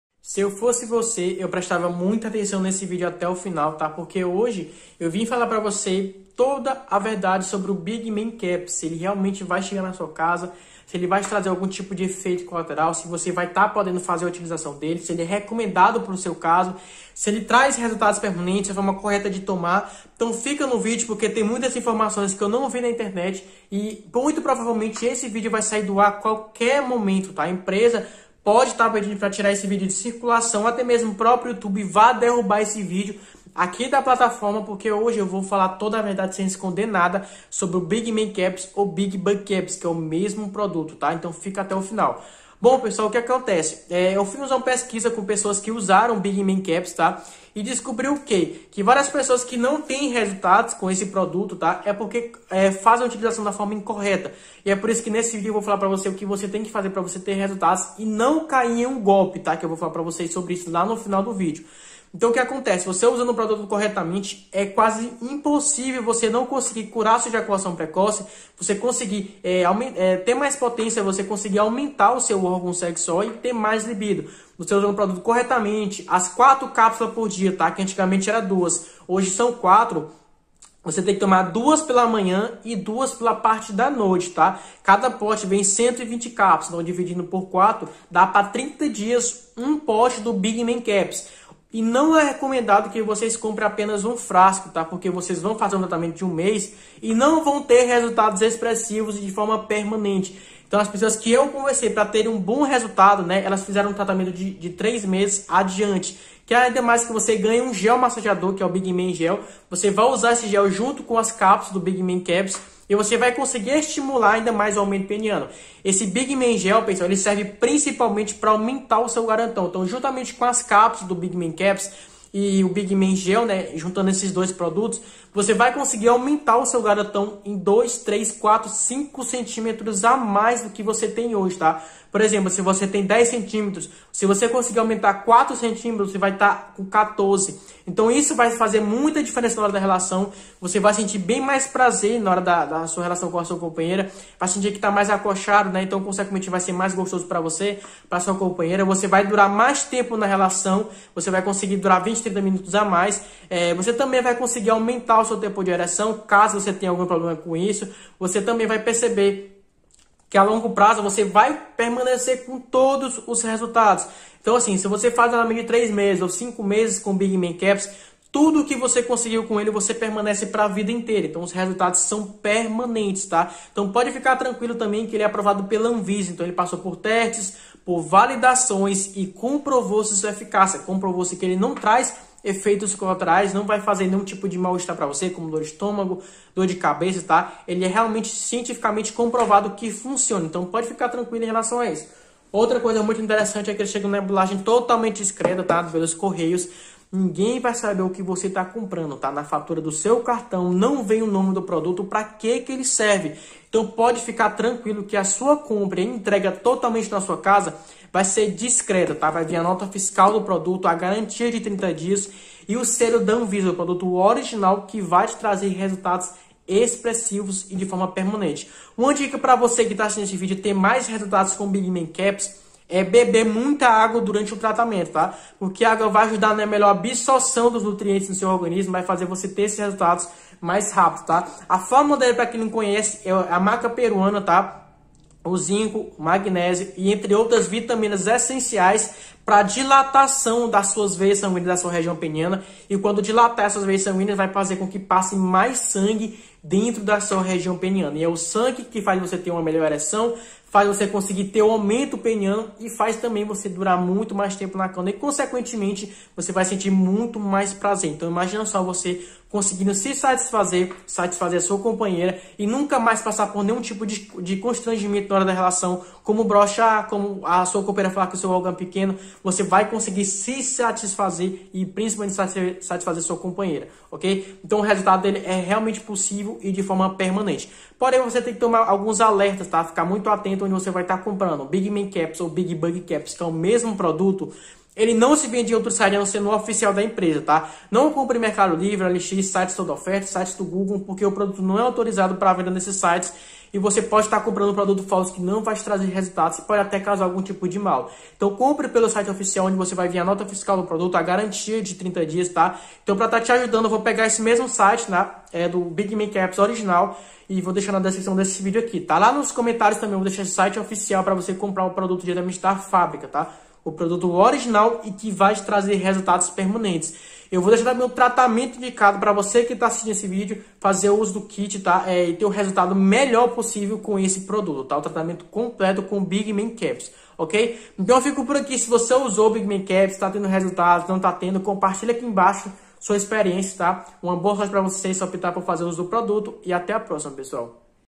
ALERTA Sobre Big Men Caps! BIG MEN CAPS Funciona Mesmo? Big Bang Caps Funciona? Big Men Caps AUMENTA
Oi, tudo bem? Vim hoje por meio desse video trazer toda a verdade sobre o Big Men Caps ou Big Bang caps. Porque tem muitas pessoas que acabam comprando esse produto e não tendo resultados.

Se você for comprar o Big Men Caps, tenha em mente que ele é um tratamento de médio a longo prazo e não vai ser do dia pra noite que você terá todos os seus benefícios. Por isso, é recomendado pelo próprio fabricante o uso do mesmo durante 3 meses pelo menos que seria de 3 potes adiante.

Até porque cada pote vem 120 cápsulas e você tomando 4 por dia, 2 de manhã e 2 pela noite vai dar pra 30 dias de uso. Para acelerar seus resultados com Big Men Caps, recomendo que você ingira muita agua ao longo do dia, pelo menos 2 litros de água. Isso vai te ajudar no hidratamento do seu corpo e melhora a absorção dos nutrientes no seu organismo.

Big Men Caps serve para:
- Combater a impotência sexual
- Ejaculação precoce
- Falta de líbido
- Aumento peniano (2 a 5cm)

Quero ressaltar que Big Men Caps ou Big Bang Caps é 100% natural e aprovado na Anvisa, ou seja, ele passou por vários testes e validações e comprovou-se sua eficácia.
Ele também possui resultados permanentes ao final do seu tratamento.

Para finalizar, quero pedir a atenção onde vai adquirir o Big Men Caps, pois o mesmo só é vendido através do site da empresa. Evite mercado livre e olx até porque é onde mais há falsificações do big bang caps.